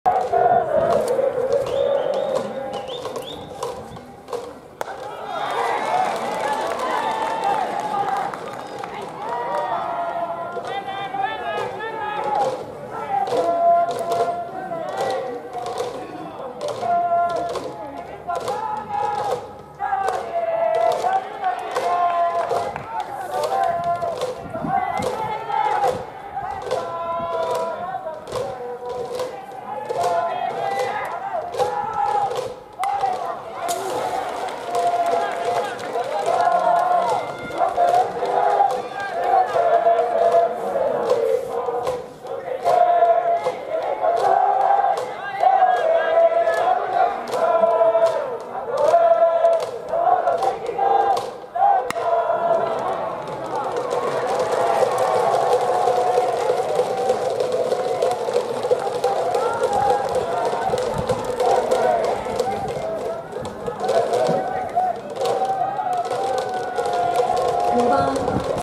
●【2018春季大会/3番打者・主将】2018/03/24尽誠学園高新3年生・犬飼 慶樹(愛知西リトルシニア)
相手投手は、藤井学園寒川高校新3年生・山岡 龍弥(大阪旭リトルシニア)
≪春6回・夏11回甲子園出場の名門私立校≫
外野手※主将　175センチ76キロ　右・左
【8回裏無死走者なし・第4打席/右前安打】
この試合は、3番右翼手でスタメン

春季四国地区高校野球香川県大会2日目
12時30分PB
2回戦vs藤井学園寒川高校@香川県丸亀市・レクザムボールパーク丸亀(両翼100m/中堅122m)
【試合結果】
寒川高5-3尽誠学園高(延長13回・タイブレーク)
■試合詳細

S2880071